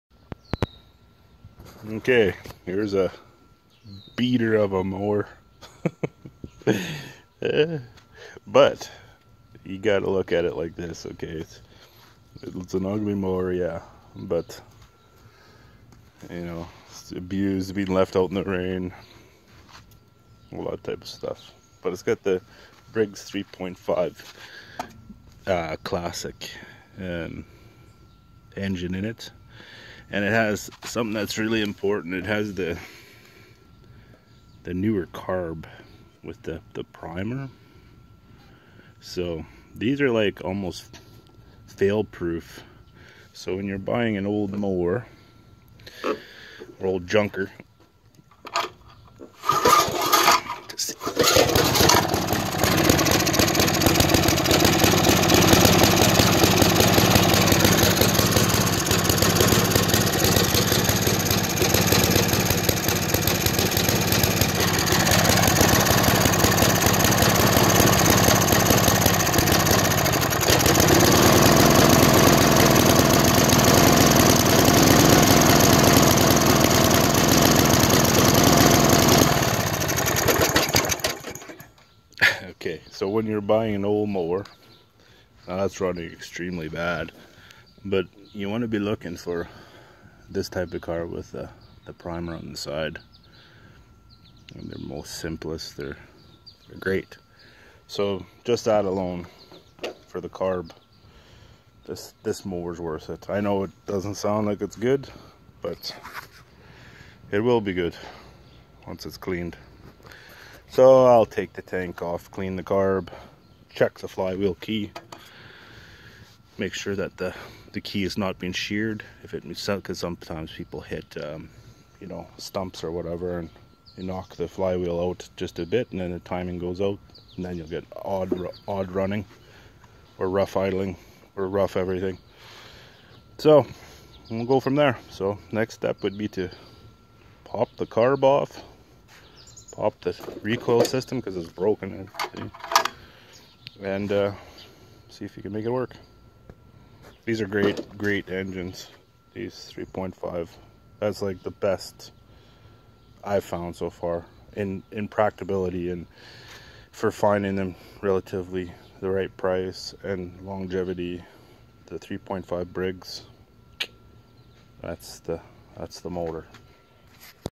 What to look for when buying a junker lawnmower!
Heres what i find to be the key to sucess when buying or finding a old lawnmower!